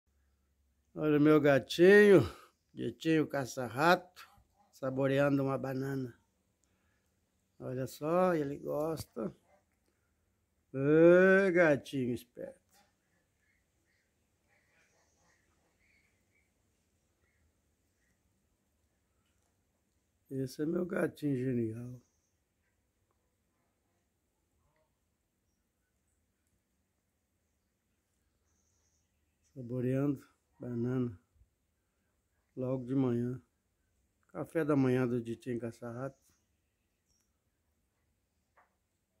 O gatinho genial, o Ditim-Caça-Rato comendo banana ...
HISTÓRIA DE UM GATINHO GENIAL!

Esse já é conhecido, mas, lhes apresento novamente.
É o meu gatinho, o Ditim-Caça-Rato,  um gatinho genial...
Mais que genial, aliás, é fenomenal...
 pois tem uma história de vida mais que especial!
E em resumo,  lhes contarei :
Uma criança o levou ao grupo da Solidariedade onde minha esposa é solidária...
- Dona Helena, a senhora quer ficar com esse gatinho?
- Senão ele vai morrer?
Era o Ditim-Caça-Rato bem pequenino, subnutrido,  com o rabo quebrado, estava todo molhado, pois chovia.
- Minha esposa que já tinha outros gatos, pensou, pensou... E levou aquele gatinho pretinho,  para nossa casa...
Eu ate então, não simpatizava com gatos, mas, jamais maltratei um.
Dei uma relutada, mas como é a esposa quem manda, aceitei o gatinho...
Um belo dia, vendo um futebol deitado no sofá,  literalmente... em dado momento o gatinho que estava feinho,  subiu no braço do sofá,  subiu na minha perna, veio caminhando e deitou na minha barriga e ficou quietinho, por horas...
Foi o motivo fundamental para que eu o " paparicasse"...
Daí,  até hoje é o meu "XODO"... o Ditim-Caça-Rato...
E fizemos uma grande parceria de amizade...
Tá ele um gatão bonito...
Bem nutrido!
Onde eu vou no sítio, ele vai e volta comigo...!
E como nós,  os animais têm  a s suas histórias.
E qual é a história do seu animal?

Essa é a história de vida do meu  gatinho GENIAL, o Ditim-Caça-Rato!!!

Romeu Borges da Silva
Amigo Ditim-Caça-Rato